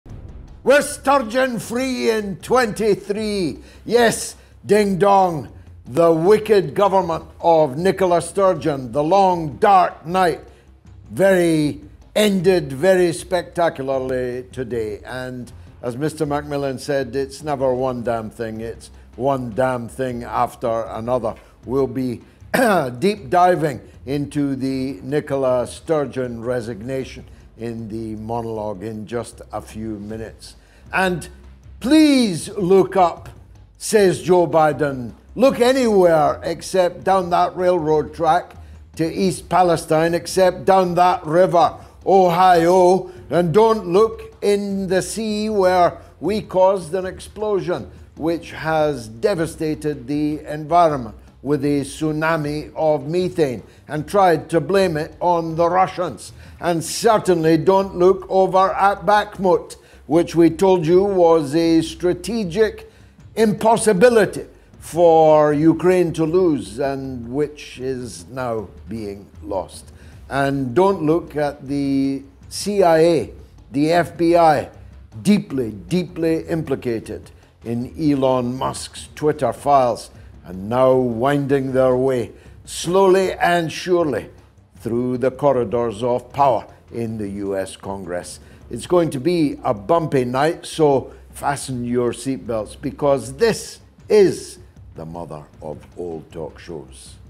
MENU: MADNESS characterised STURGEON’S reign
"MENU: From Edinburgh to East Palestine to Nord Stream, toxic clouds and lies. And in Ukraine it’s the fog of war or is it gun smoke?

Follow MOATS on YouTube Sturgeon Ukraine"